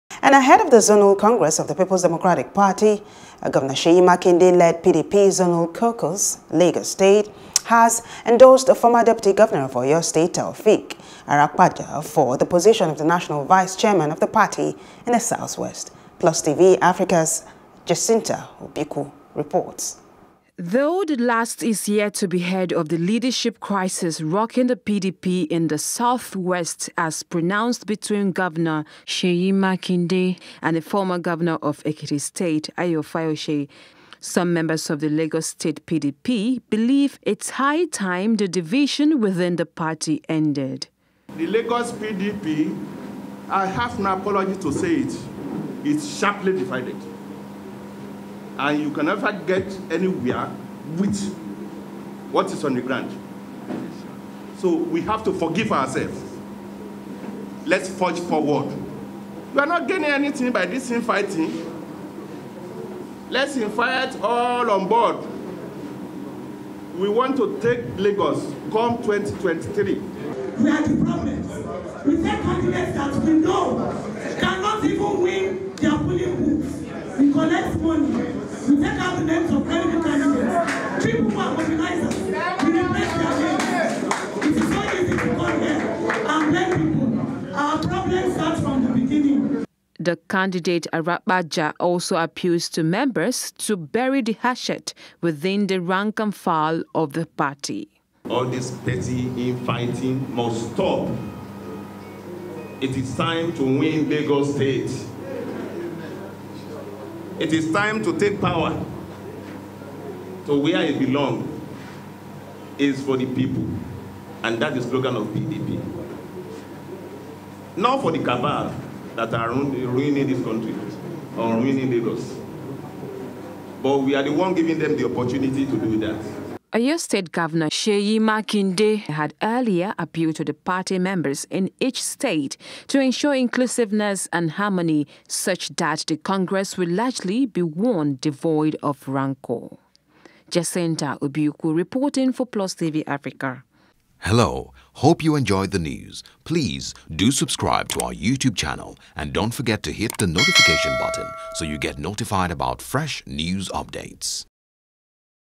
PDP Endorses Arapaja Candidacy as National Vice Chairman | NIGERIA
Lagos PDP Endorses Arapaja Candidacy as National Vice Chairman.